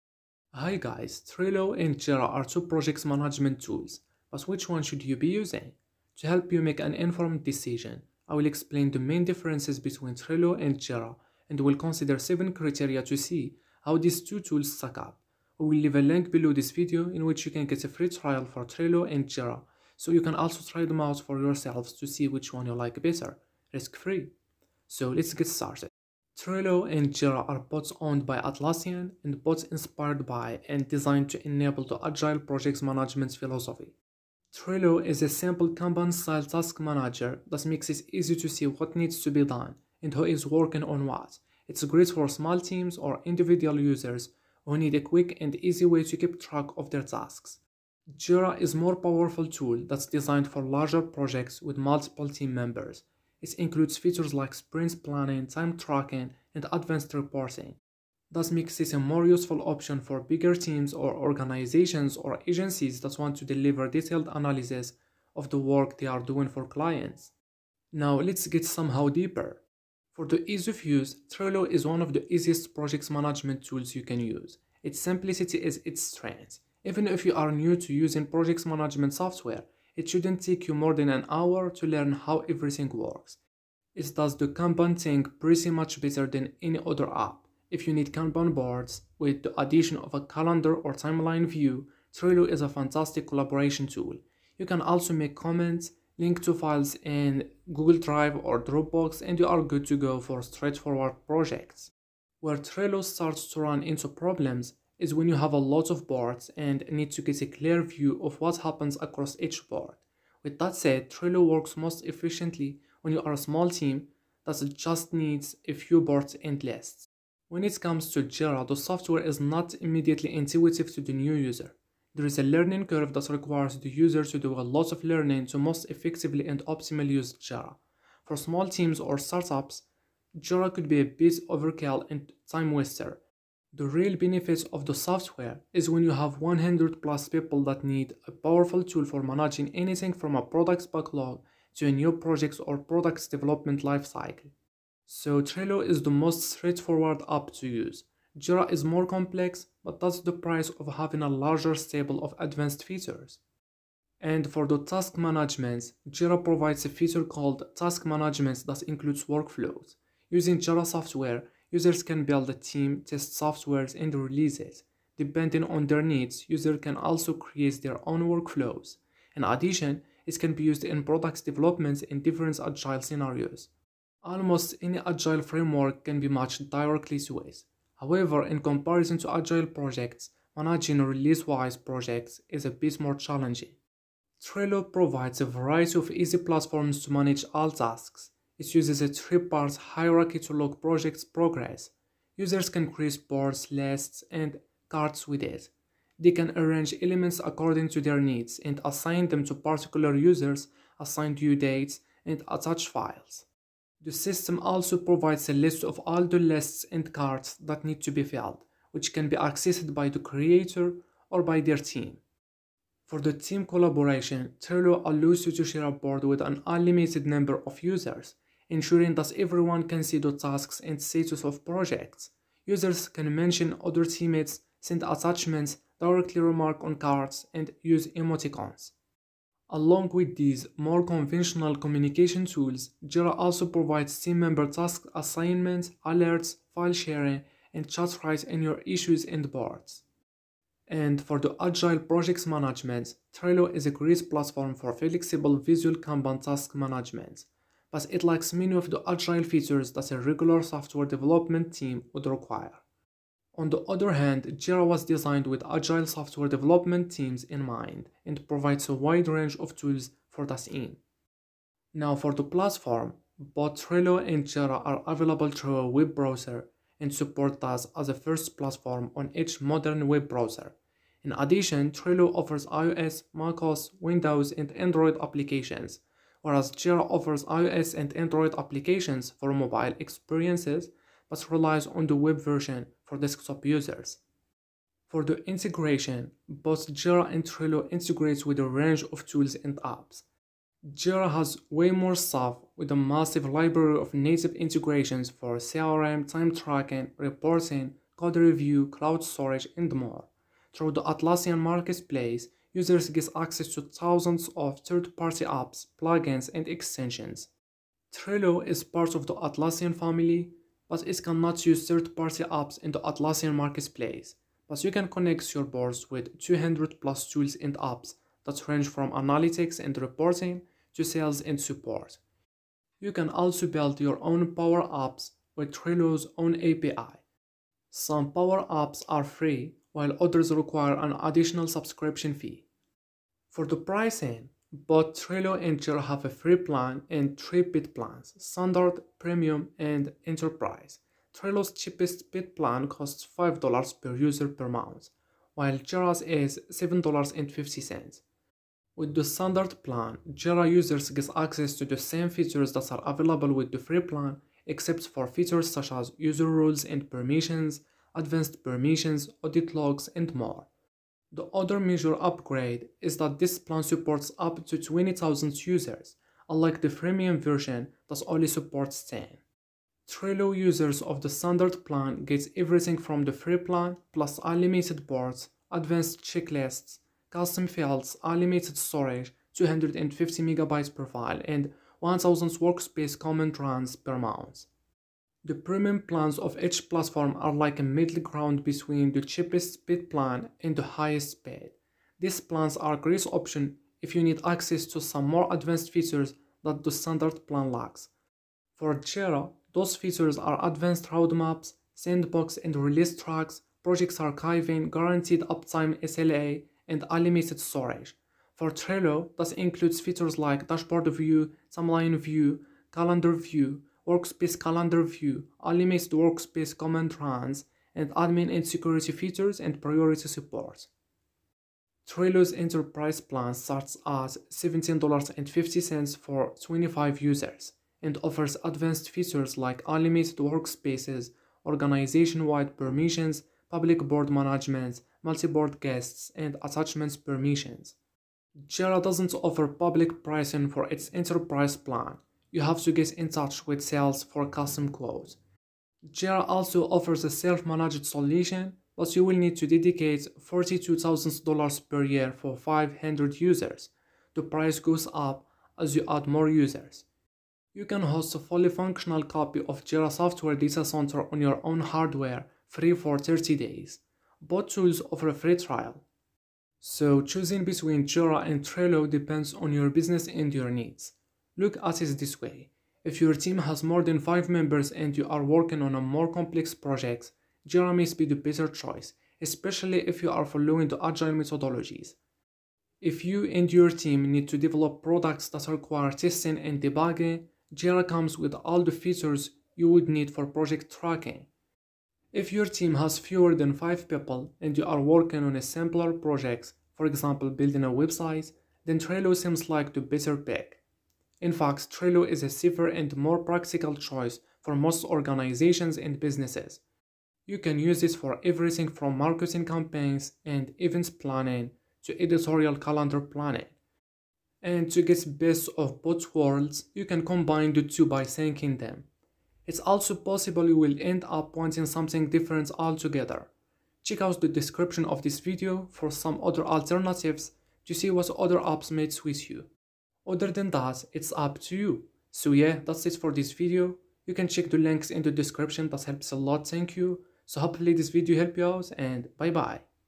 Jira vs Trello - Which One Is Better?
In this video you will find out Which One is Better - Jira or Trello

🔥 Special Offers 🔥

Chapters:
0:00 Intro
0:26 What is Trello vs Jira
1:10 Ease of use
2:33 Task Management
3:31 Team Collaboration
4:00 Agile PM
4:21 Platforms
4:44 Integration
5:32 Pricing
7:46 Conclusion